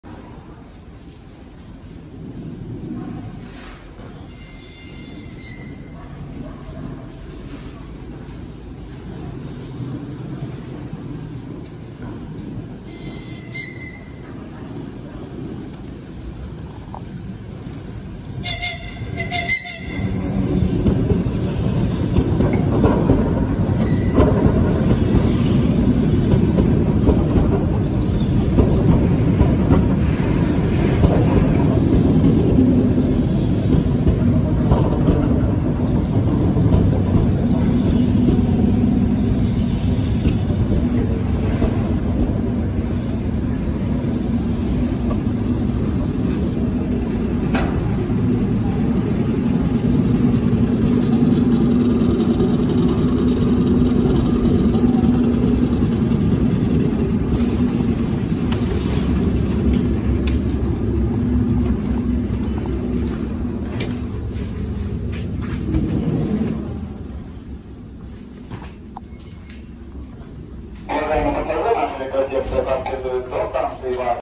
Прибытие ЭР2К-980 «Лахта» с приветливой бригадой на станцию Девяткино|| 500 видео на канале)
Прибытие ЭР2К-980 «Лахта» с приветливой бригадой на станцию Девяткино|| 500 видео на канале, поздравляю)

Место съемки: Россия, Ленинградская область, станция Девяткино

Сведения о электропоезде

Дорога приписки: Октябрьская железная дорога
Депо приписки: ТЧ-15 Санкт-Петербург-Балтийский
Серия: ЭР2к-980
Завод-изготовитель: Рижский вагоностроительный завод  СССР Рига
Построен: 08.1972
Категория: Электропоезда
Текущее состояние:  Эксплуатируется
Примечание: «Лахта»

Именное название:
05.2020 — присвоено именное название "Сочи"
11.2020 — присвоено именное название "Яхрома"
09.2022 — присвоено именное название "Лахта"

Происшествия:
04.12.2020 — вагоны 09 и 10 сгорели в депо Санкт-Петербург-Балтийский